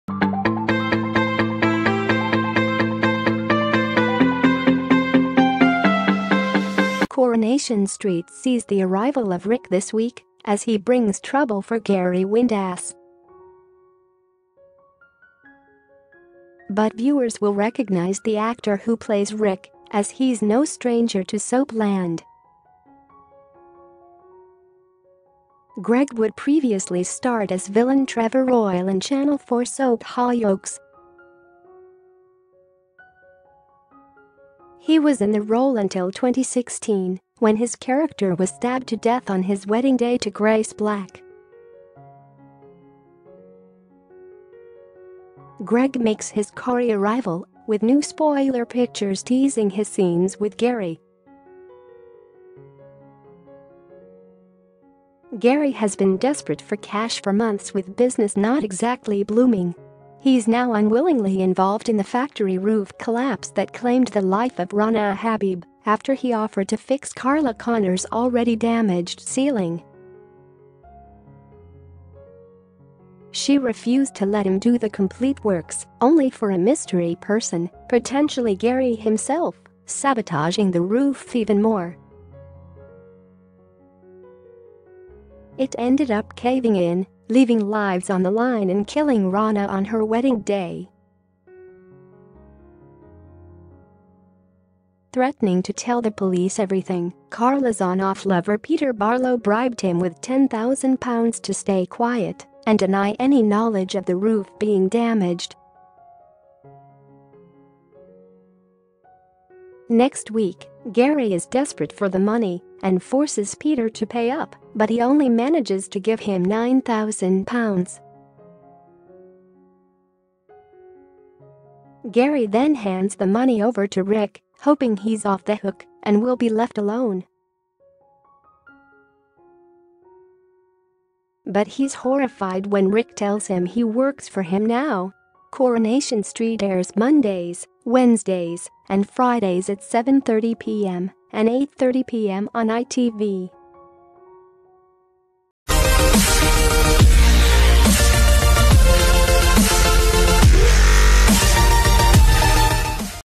Why you recognise Coronation Street's Rick as past soap role exposed - News 247
Coronation Street sees the arrival of Rick this week, as he brings trouble for Gary Windass.  But viewers will recognise the actor who plays Rick, as he’s no stranger to soap land.  Greg Wood previously starred as villain Trevor Royle in Channel 4 soap Hollyoaks.  He was in the role until 2016, when his character was stabbed to death on his wedding day to Grace Black.    Greg makes his Corrie arrival, with new spoiler pictures teasing his scenes with Gary.  Gary has been desperate for cash for months with business not exactly blooming.  He’s now unwillingly involved in the factory roof collapse that claimed the life of Rana Habeeb, after he offered to fix Carla Connor’s already damaged ceiling.   She refused to let him do the complete works, only for a mystery person, potentially Gary himself, sabotaging the roof even more.  It ended up caving in, leaving lives on the line and killing Rana on her wedding day.  Threatening to tell the police everything, Carla’s on/off lover Peter Barlow bribed him with £10,000 to stay quiet and deny any knowledge of the roof being damaged.   Next week, Gary is desperate for the money and forces Peter to pay up, but he only manages to give him £9,000.  Gary then hands the money over to Rick, hoping he’s off the hook and will be left alone.  But he’s horrified when Rick tells him he works for him now.  Coronation Street airs Mondays, Wednesdays and Fridays at 7:30pm and 8:30pm on ITV.   Why you recognise Coronation Street's Rick as past soap role exposed  - News 247